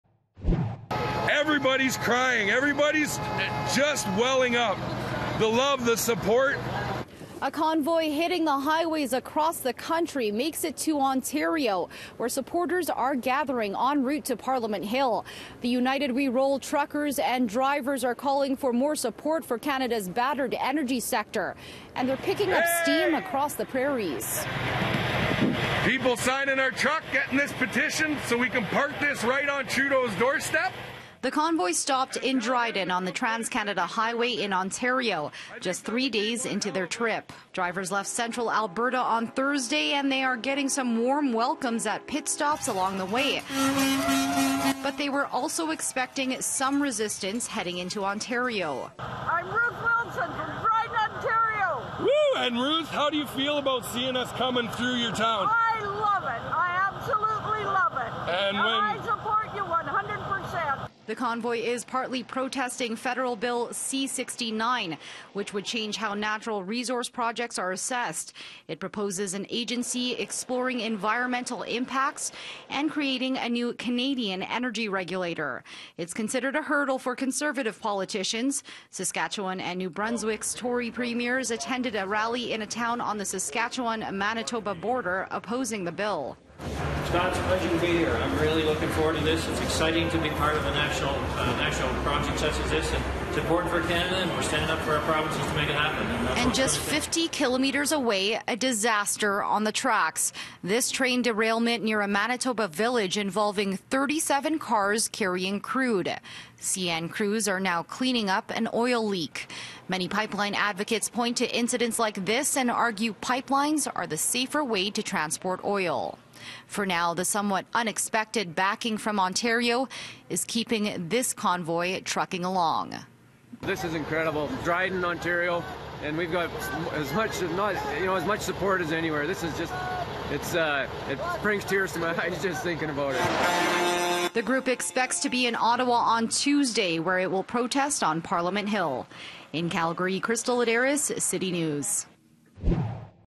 Convoy reaches Ontario
A convoy making it’s way to Parliament Hill reaches Ontario as it continues its trek to call for action to support Canada’s energy industry.